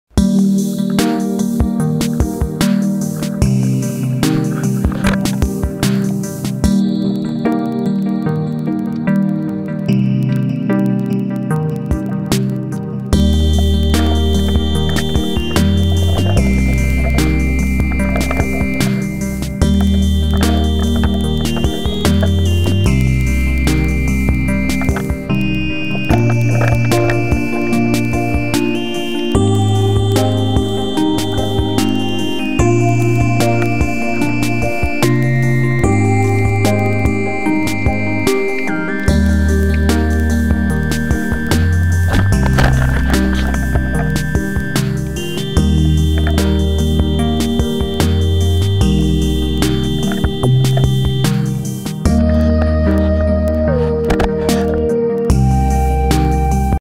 Qulche pesce di novembre....Ma con tanta voglia di mare......
Qualche cattura di novembre in un mare deserto. Con la cam che dimentico di accendere. Appena entro in acqua un bel mugo e nel finale un sarago. Ma tutto sommato sono entrato in acqua. Un saluto a tutti...